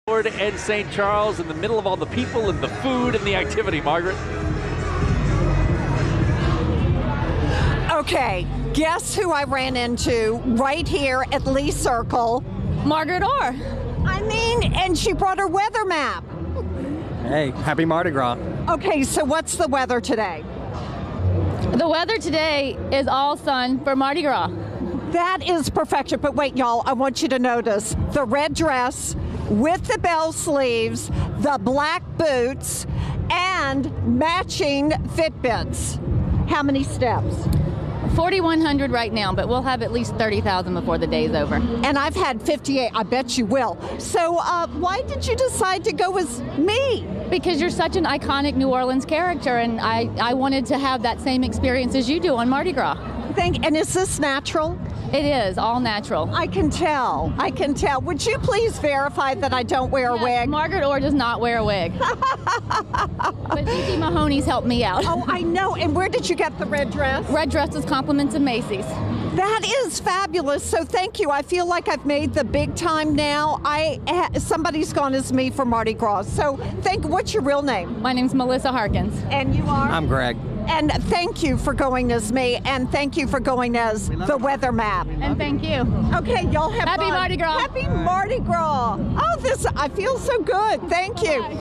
Margaret Orr finds her doppelgänger!
Margaret Orr found someone dressed up as herself for MardI Gras 2018!